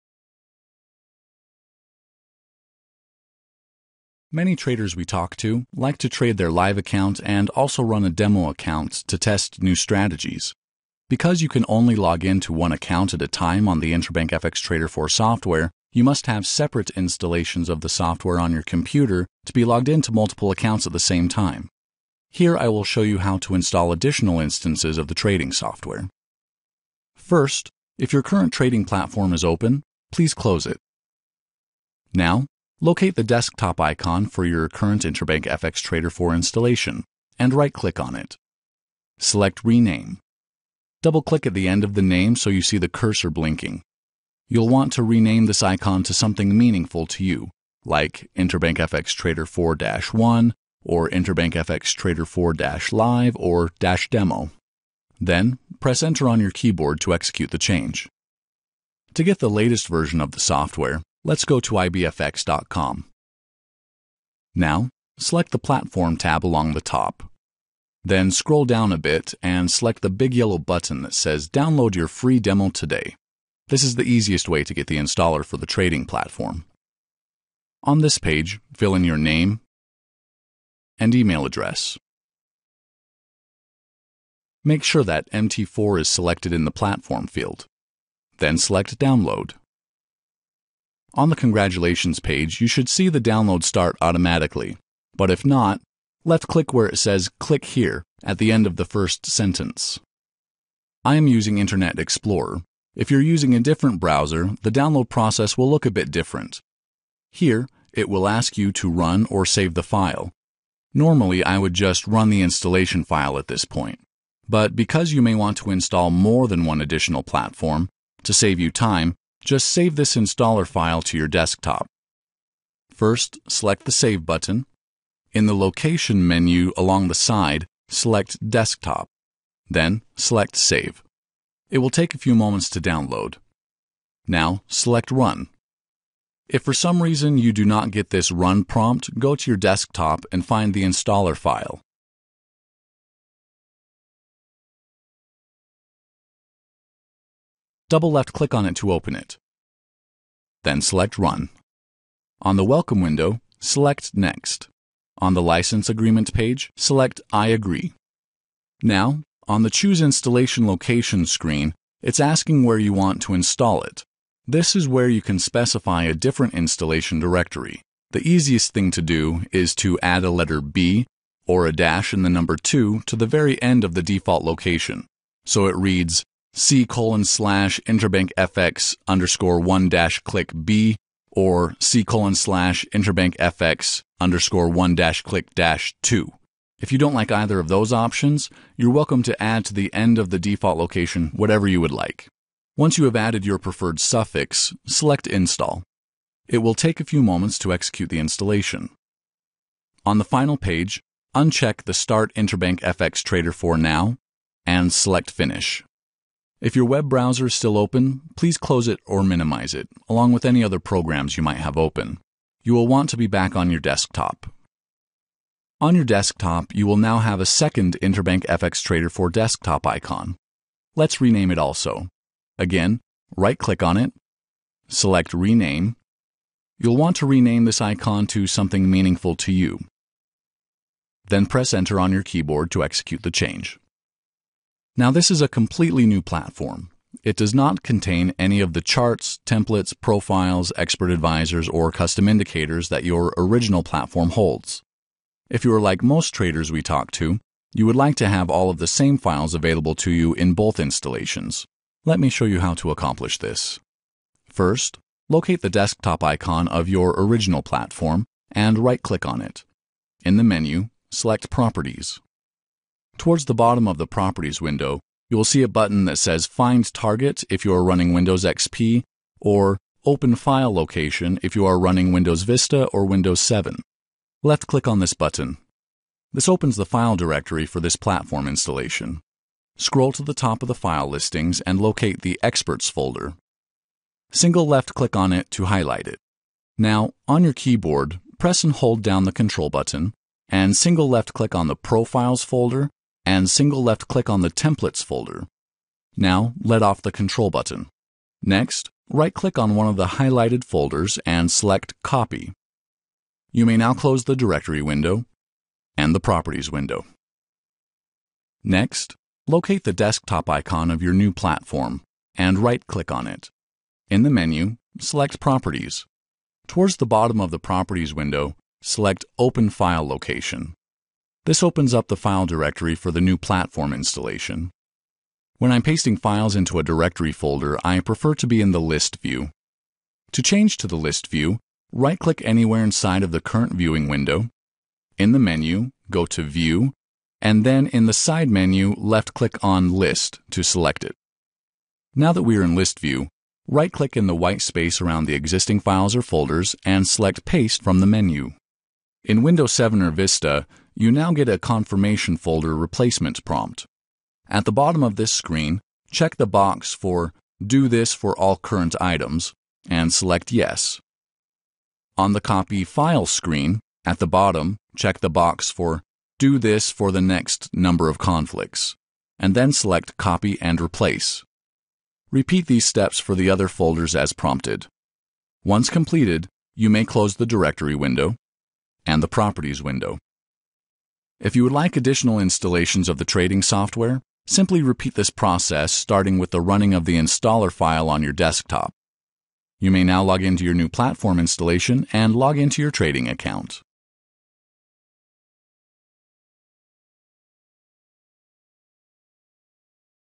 IBFX - How to Install Additional Platforms
Many traders we talk to like to trade their live account, and also run a demo account to test new strategies. Because you can only login to one account at a time on the Interbank FX Trader 4 software, you must have separate installations of the software on your computer to be logged into multiple accounts at the same time.  Here we will show you how to install additional instances of the trading software.